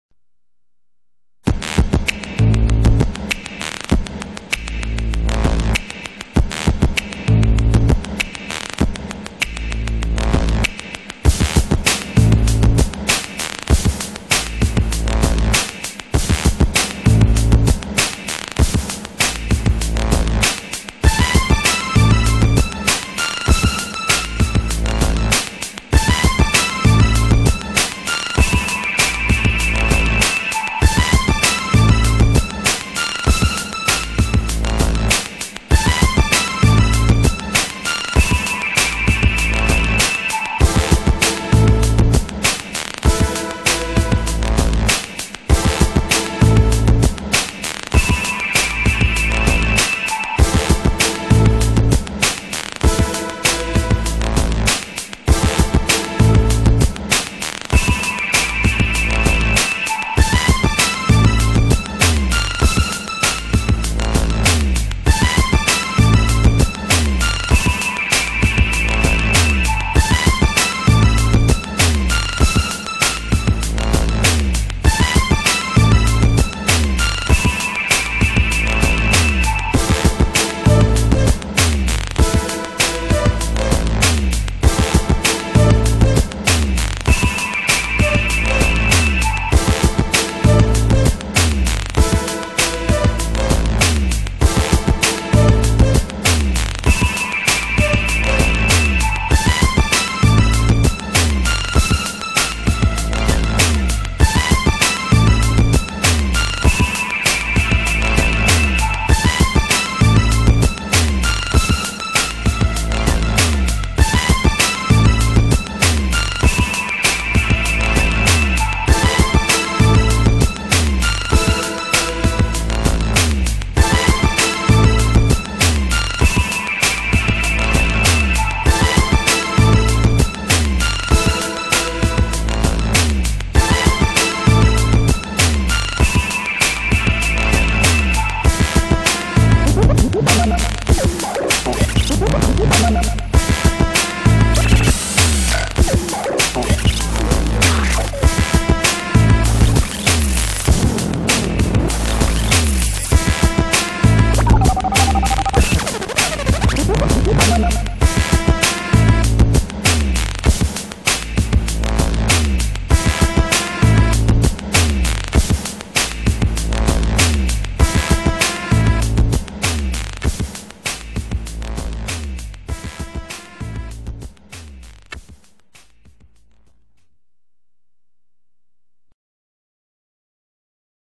SAGAR'S - Living Head Of Decapitated Dog.flv
Real video showing Soviet scientist maintaining the life of a decapitated dog's head, etc. To listen and download free music from The Relative It